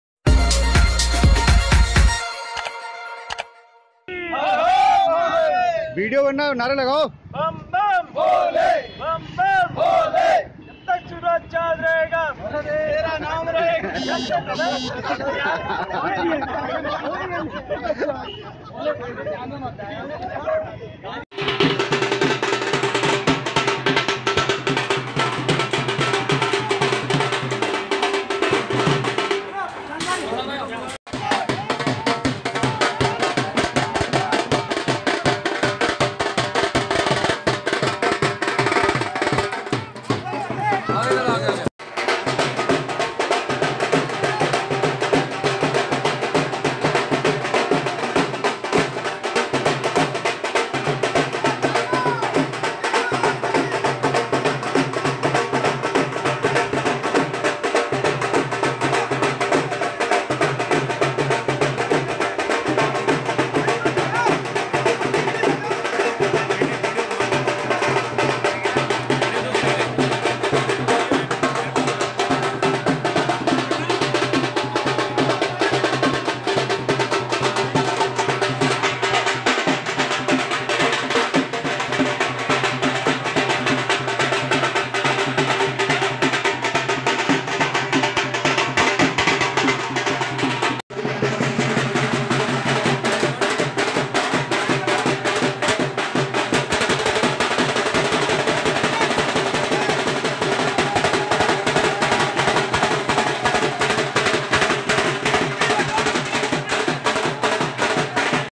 Baba Barfani Amarnath Jate samay Hindu Mahasabha dwara bhabhi Swagat
⛳⛳जय बाबा बर्फानी अमरनाथ जाते समय भब्य विदाई समारोह हिन्दू महा सभा द्वारा जय श्री राम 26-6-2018 मग्गल वार ⛳⛳